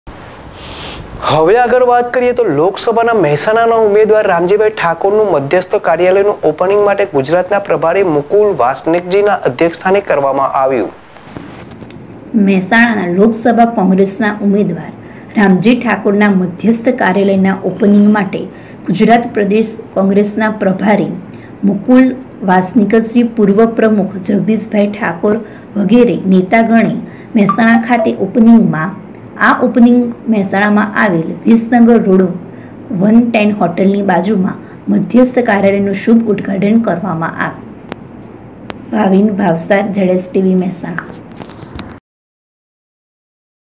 મહેસાણાના ઉમેદવાર રામજીભાઈ ઠાકોર નું મધ્યસ્થ કાર્યાલયનું ઉદઘાટન માટે ગુજરાત કોંગ્રેસના પ્રભારી
મહેસાણાના ઉમેદવાર રામજીભાઈ ઠાકોર નું મધ્યસ્થ કાર્યાલયનું ઉદઘાટન માટે ગુજરાત કોંગ્રેસના પ્રભારી મુકુલ વાસનિકજી ના અધ્યક્ષ સ્થાને કરવામાં આવ્યું હતું